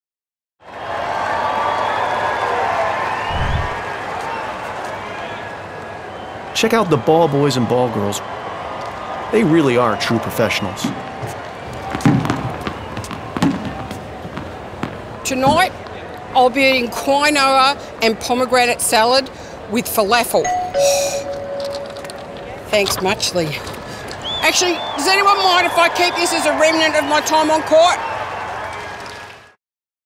Uber Eats Sharon Strzelecki AO TVC via Special Group
Not everyone can watch the greatest tennis players in the world live from the Rod Laver Arena during the Australian Open. So the team at Uber Eats and creative agency Special Group have done it again and created more great moments that will serve as a reward for those catching the action at home.